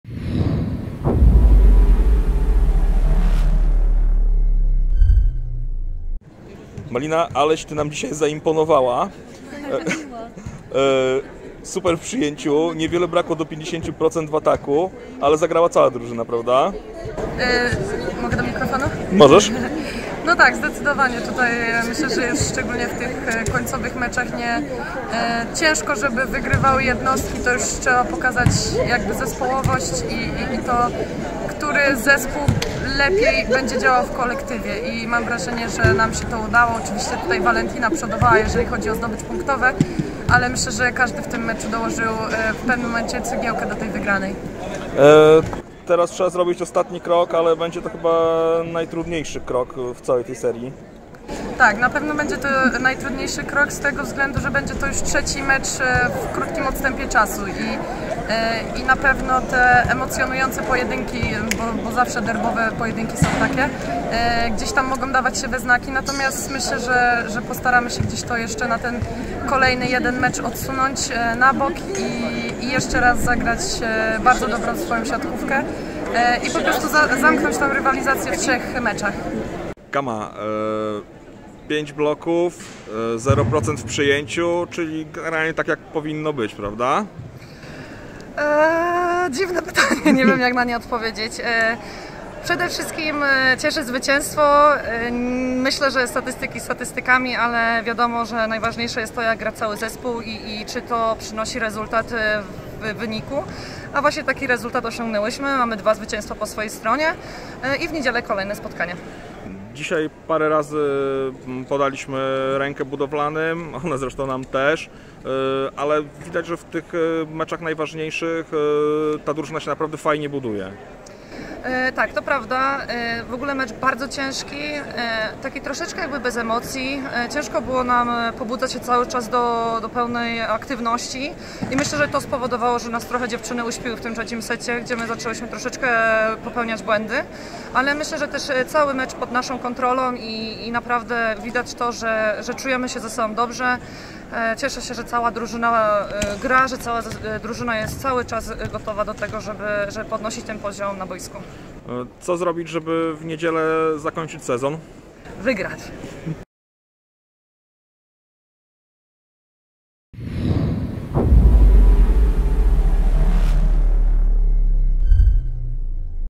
Martyna Grajber i Kamila Witkowska po 2. meczu o brązowy medal MP
- W tych końcowych meczach ciężko, żeby wygrywały jednostki. To już trzeba pokazać zespołowość - podkreśliła po 2. meczu o brązowy medal Martyna Grajber.

- Myślę, że widać to, że czujemy się ze sobą dobrze - dodała Kamila Witkowska.

Zobaczcie i posłuchajcie jak 2. mecz derbowy o brązowy medal MP skomentowały siatkarki ŁKS-u Commercecon Łódź.